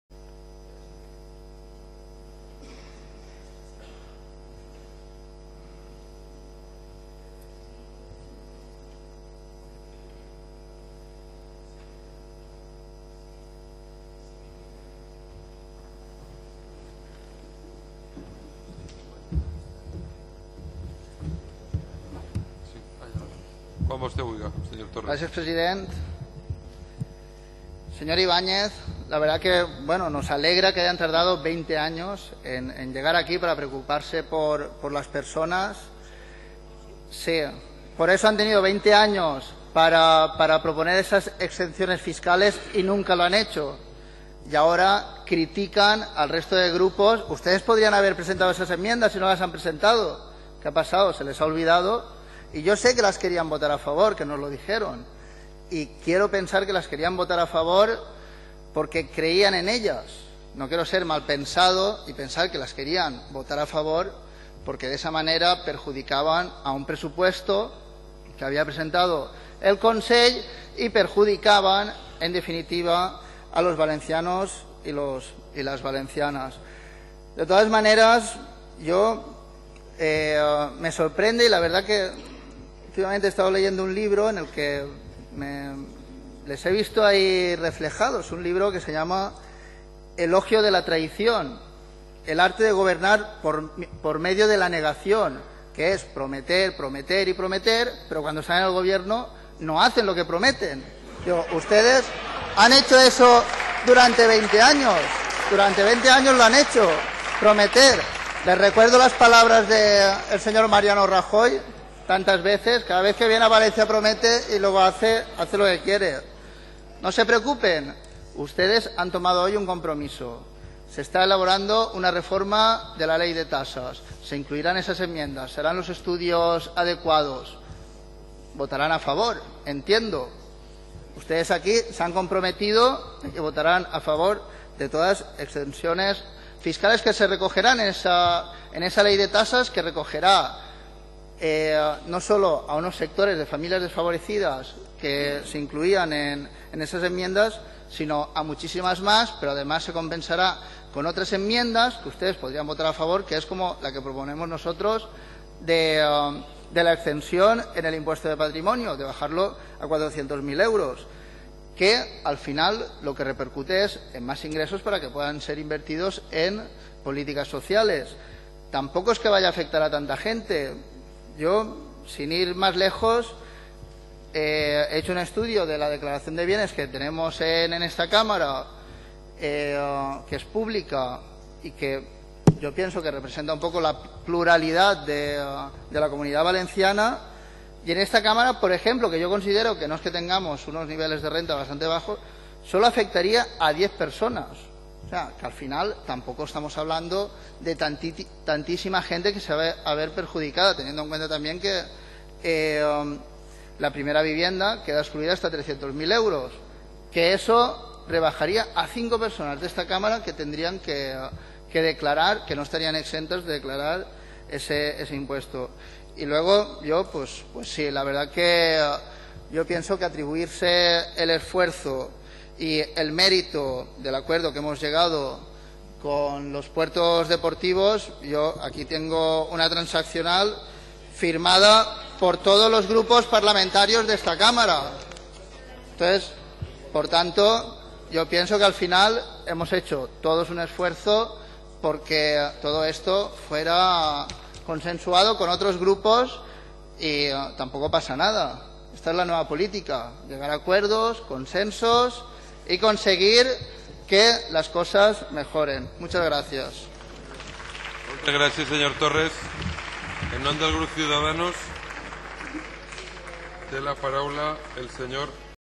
David Torres: "Se recupera el impuesto de patrimonios para ampliar las políticas sociales"
David Torres (portavoz de economía, Podemos) interviene en el Ple de les Corts sobre la Ley de Acompañamiento y fiscalidad a los presupuestos de 2016.